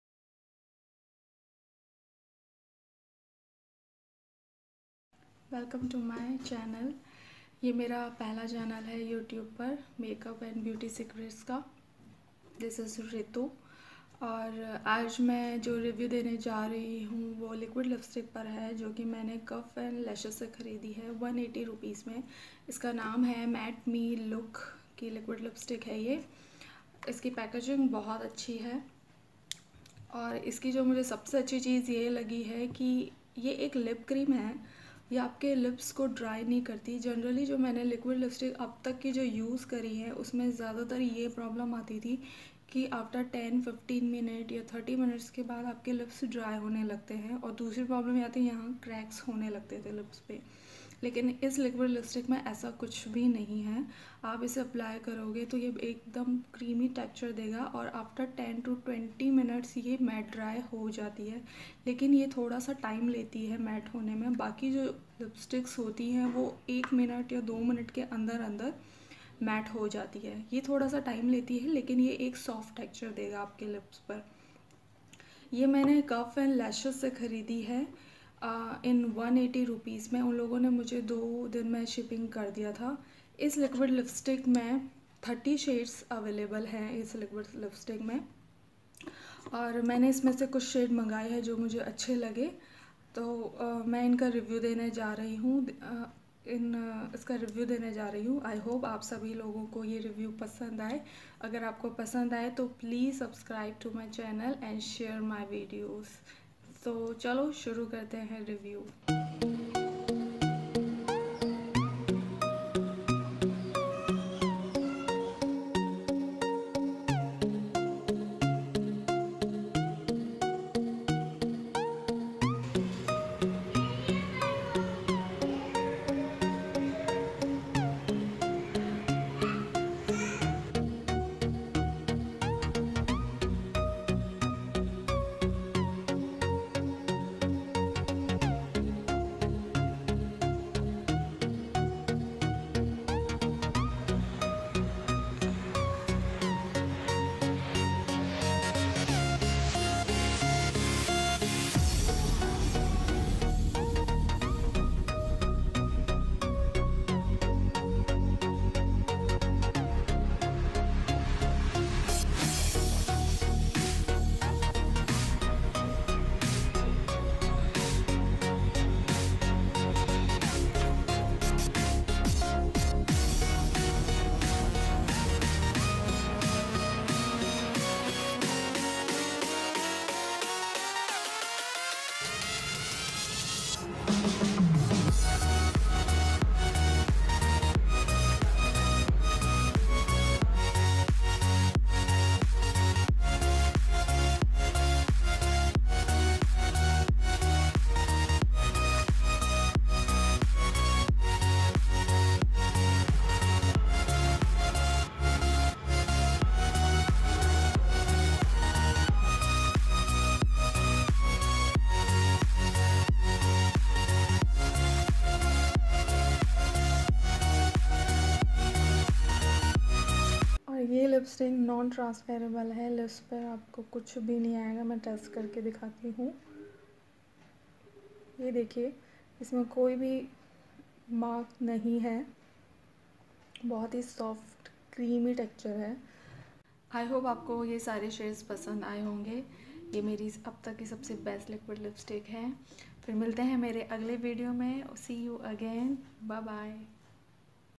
Matt liquid lipsticks reviews|| By Beautymaniya with Ritu
Hello everyone this is my first video on youtube channel . I hope you all are doing very  wel.  In this video i m giving the review on" MATT LOOK Super Stay '' liquid lipstics this liquid  lipstics i bought from
(cuffsnlashess .com) website.
thanx for watching
Beautymania with Ritu
Beautymaniawithritu